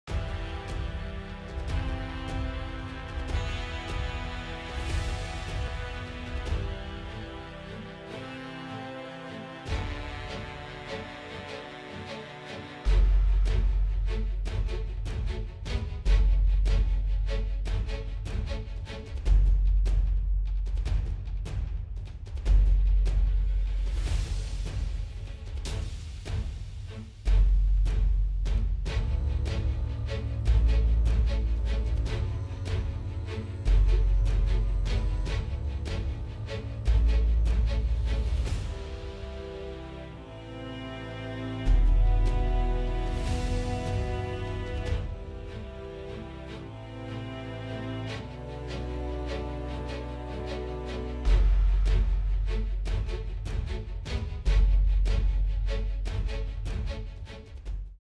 Two polar bears battle over a seal carcass. Life is tough in the Arctic!
Two polar bears vie for a seal carcass. Getting food is tough in the Arctic! On the ice near Svalbard, this large male polar bear had killed a beared seal and you can see from the state he is in that he had been feasting on the kill for some time. The younger male tried several times to sneak a mouthful but was chased off each time. He did manage to get some food but the larger male prevailed. Fantastic encouter.